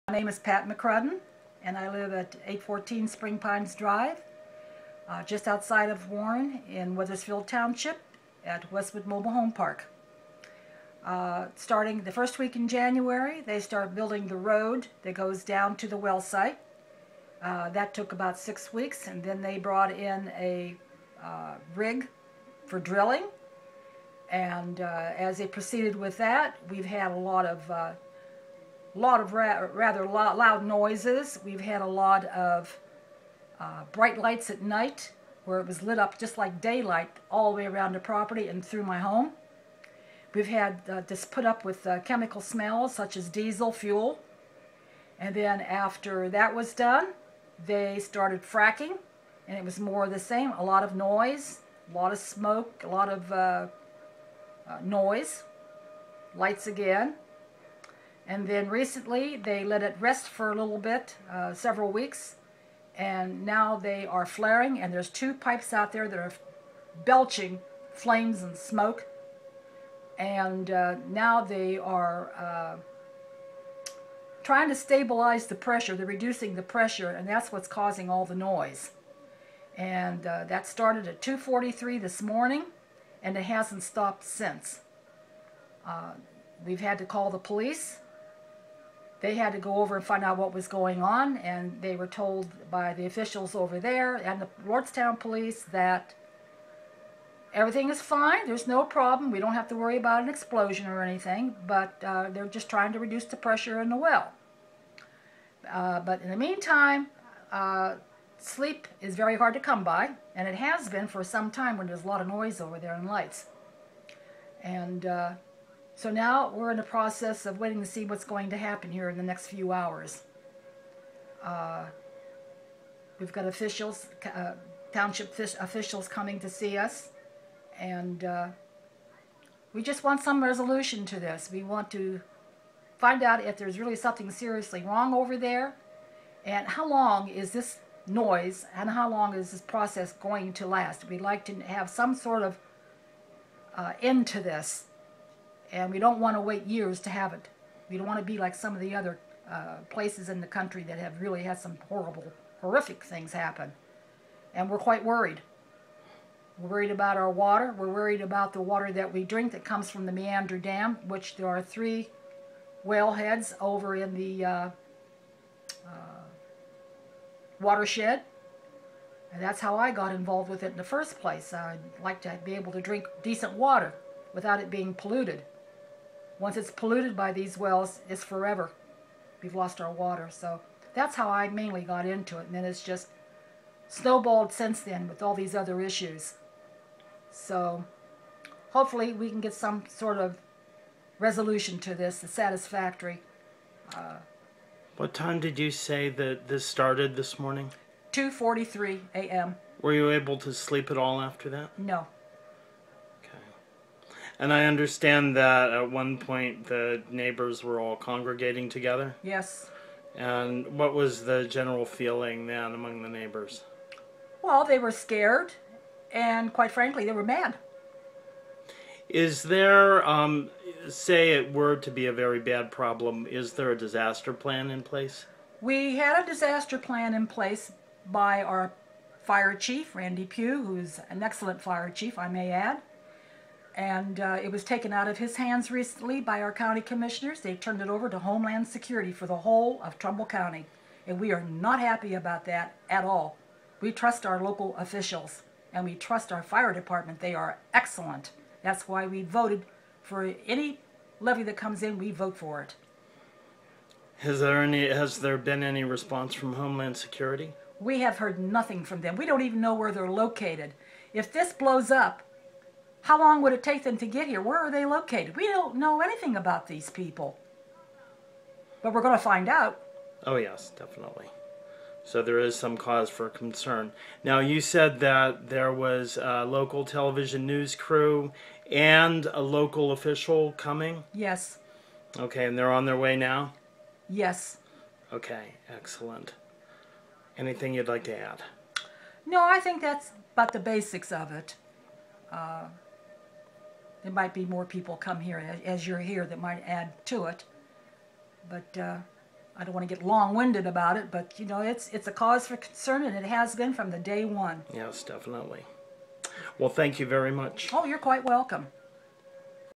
Pat McCrudden, Weathersfield, OH, Fractivist | Occupy Oral History
Pat McCrudden discusses living next to a hydrofracturing site, the current conditions, and her intention to continue to work for safe conditions for herself and her neighbors.